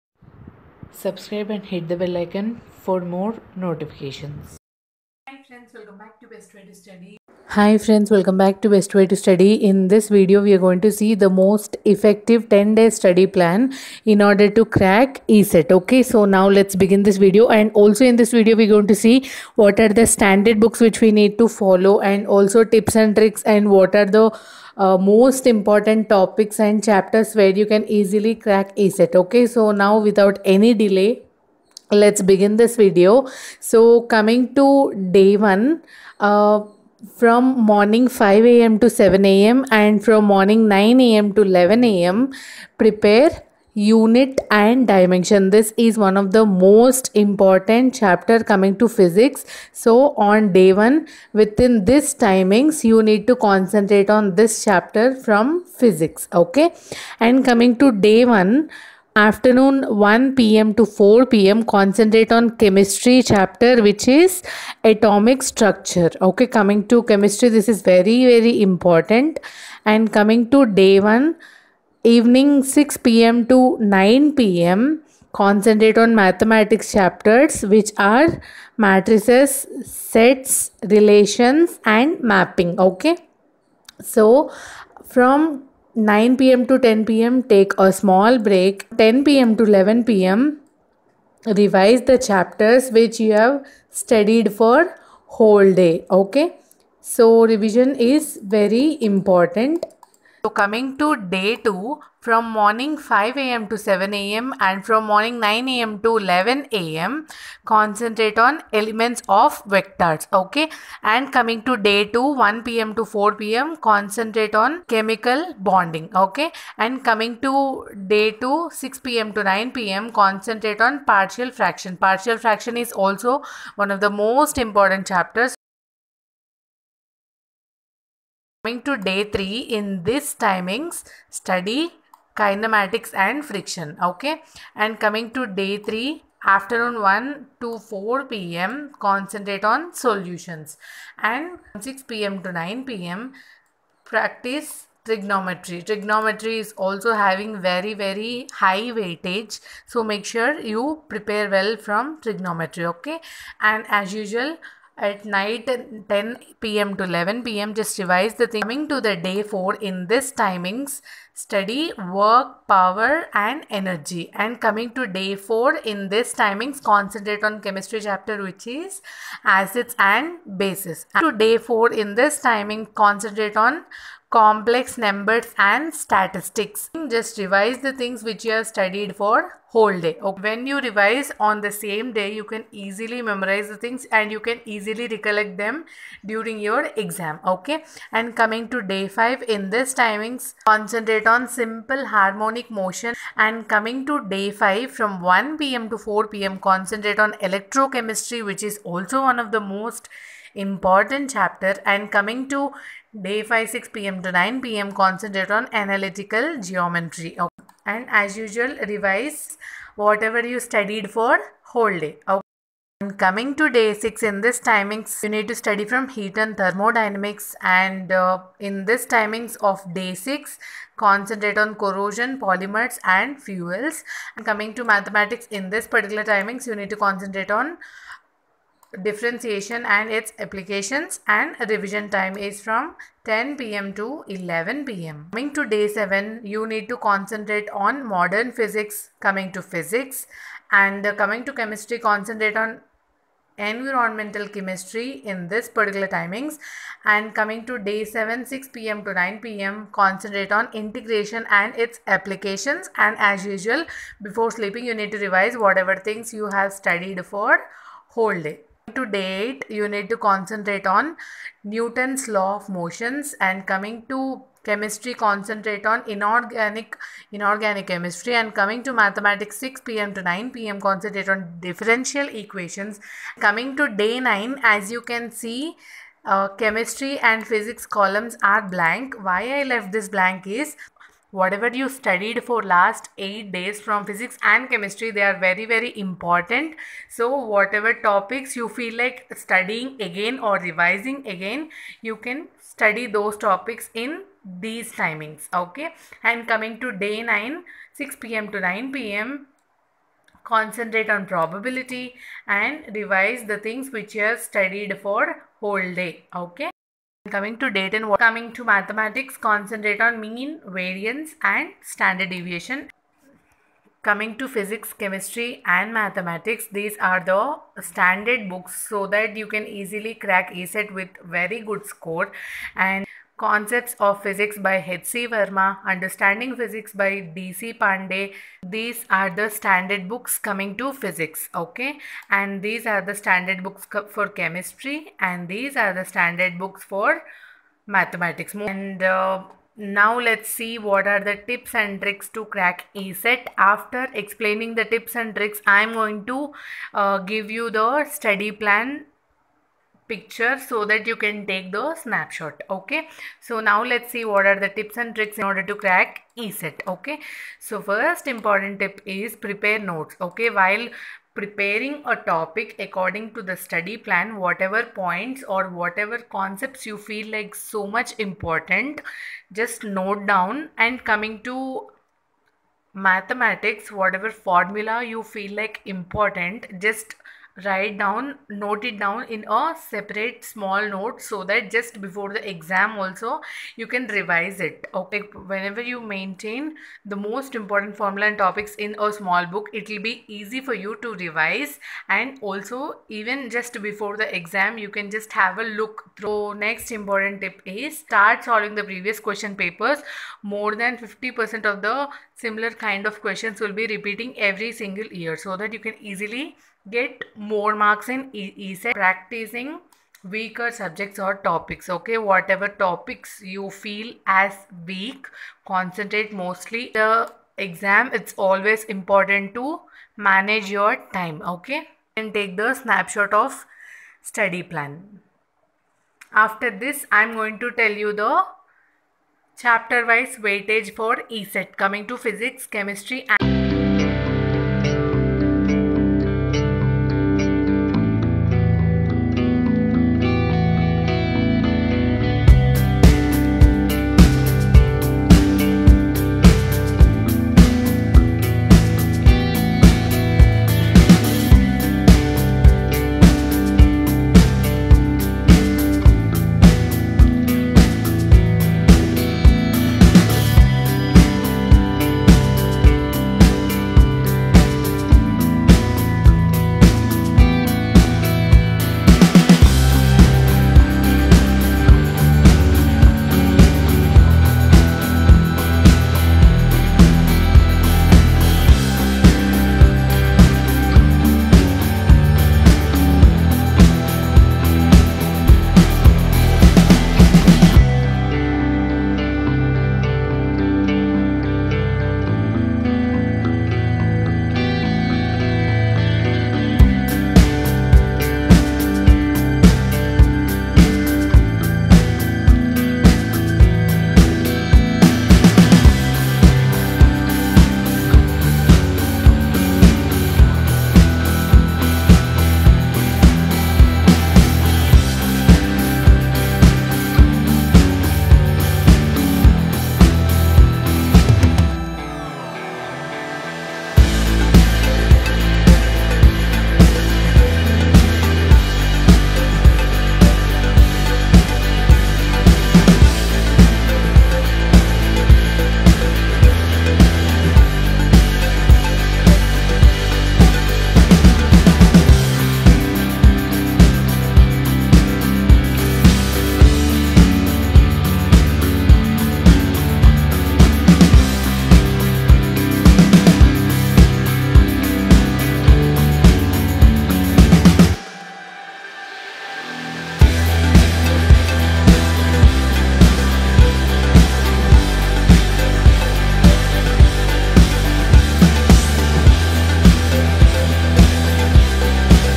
10 DAYS STUDY PLAN TO CRACK ECET || Which books to follow? || Most important topics for ECET || BWTS
How to prepare perfect ecet preparation plan in 2020 || ECET PREPARATION IN 2020

ECET is the Entrance examination which is conducted by JNTU (Jawaharlal Nehru Technological University Anantapur) Every Year on behalf of Andhra Pradesh State Council of Higher Education. This examination is conducted for taking admission into various Undergraduate courses.